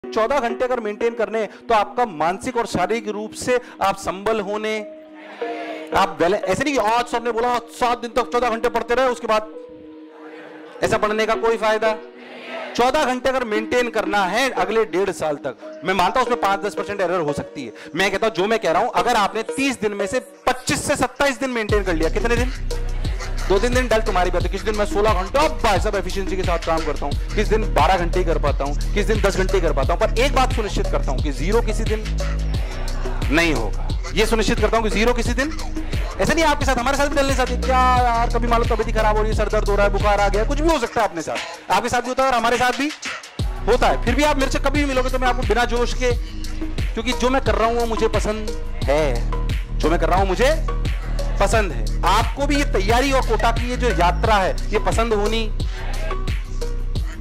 Selection चाहिए तो आदतें अच्छी डालो 🔥| NV Sir Shorts |Motion JEE
Kota is a place that is not only considered an educational hub but also teaches students a lot of things, such as consistency and hard work with efficiency. Watch this video to know what NV Sir has to say about the hard work and efficiency required for not only clearing their desired exam but also being successful.